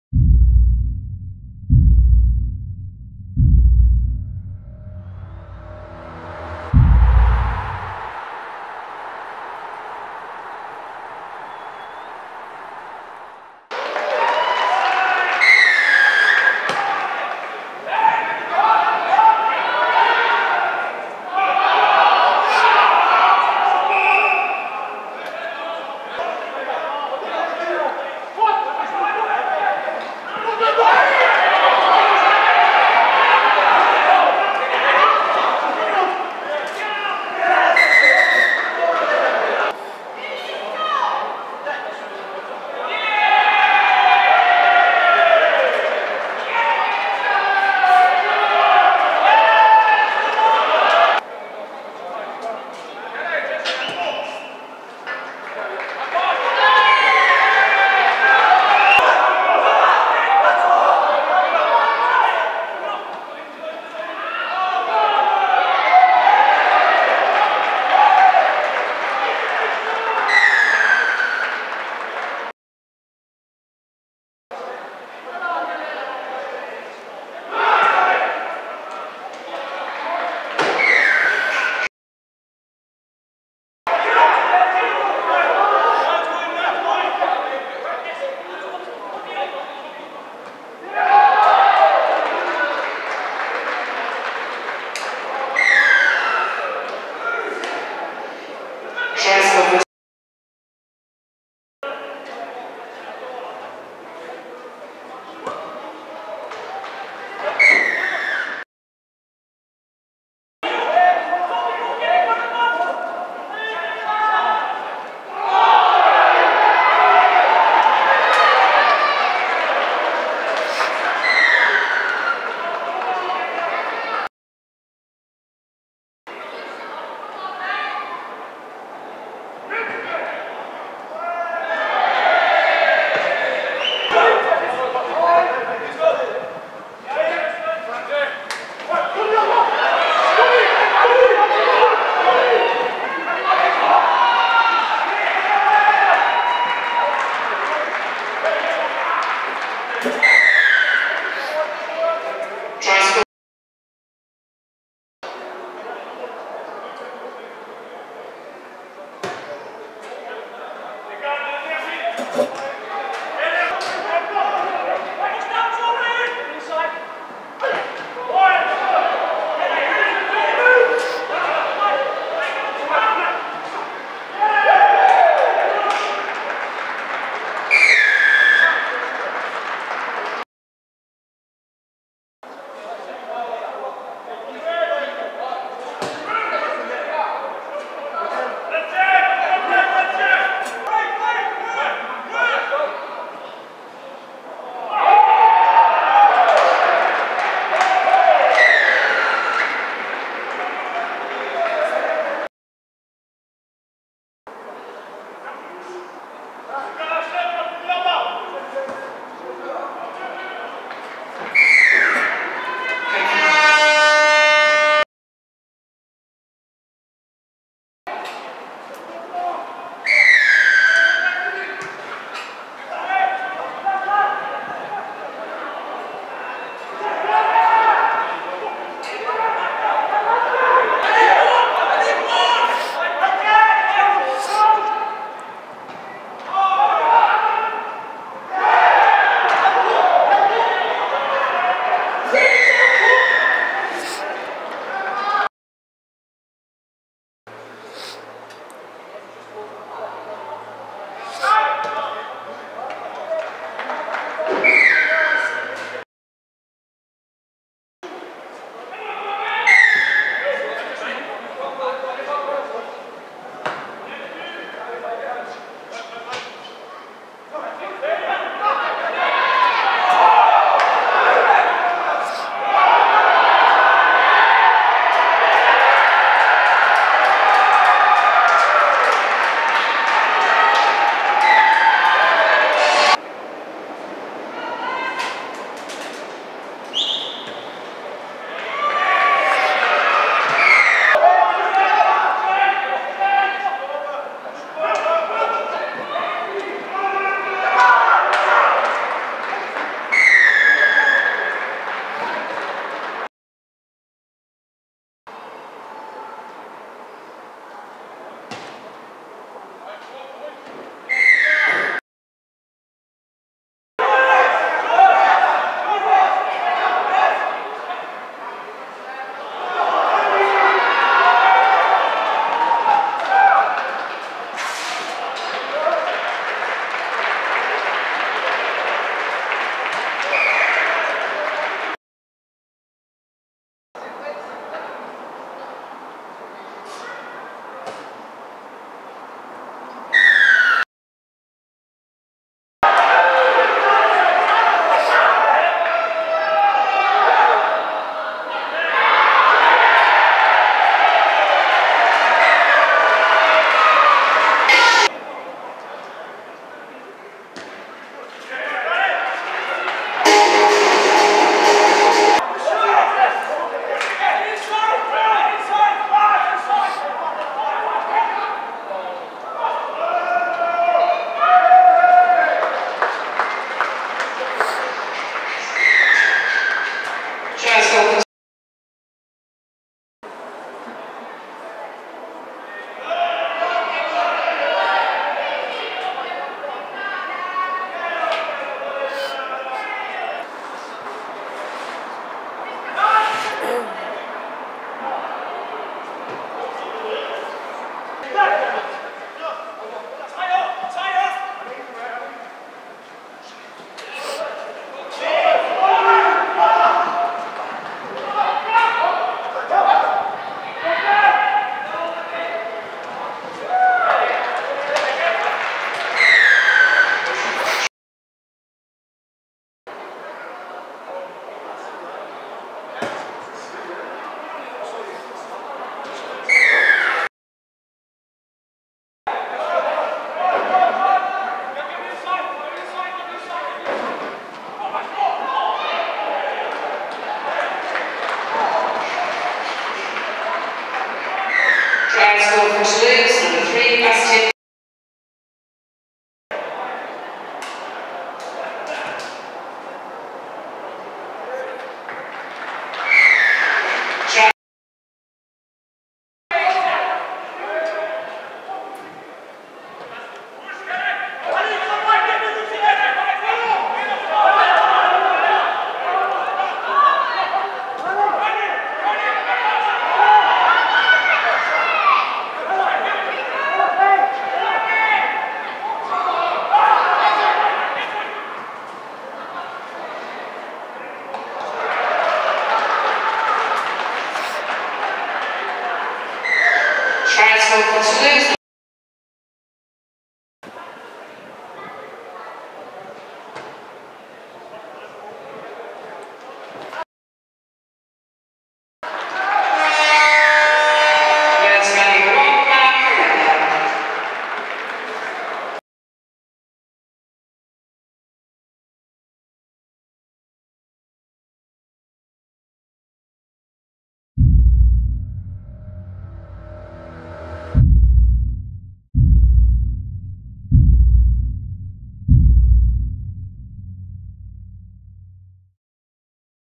Résumé Rochdale v TO XIII - Round 18 Championship - 24.06.2018
Le Toulouse Olympique a signé sa plus grosse victoire de la saison ce dimanche (24/06) sur la pelouse de Rochdale (14-70).

13 essais marqués et une prestation aboutie pour un succès important puisque l’ensemble des concurrents du haut de tableau l’ont aussi emporté sans contestation. Le TO conserve ainsi sa deuxième place au classement, et se prépare pour les 5 derniers matchs de la première phase.